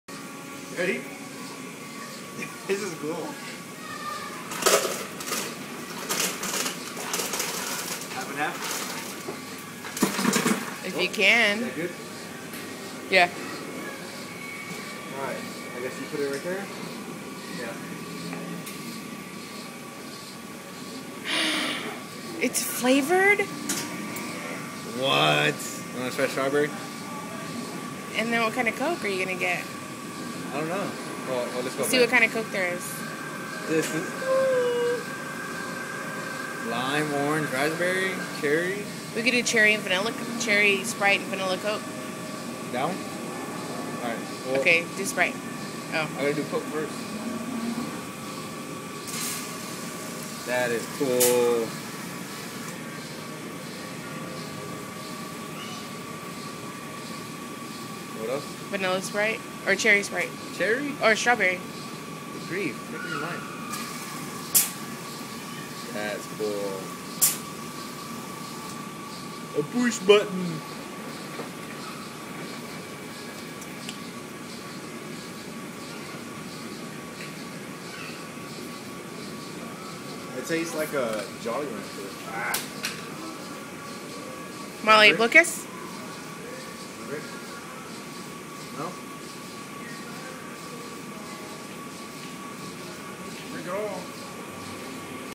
Awesome Coke Machines!!!
We are in San Antonio right now and we brought Molly to see Rango at the Santikos Palladium at the Rim & we saw these Coke Machines and Geek'd out! Lol how awesome are they!!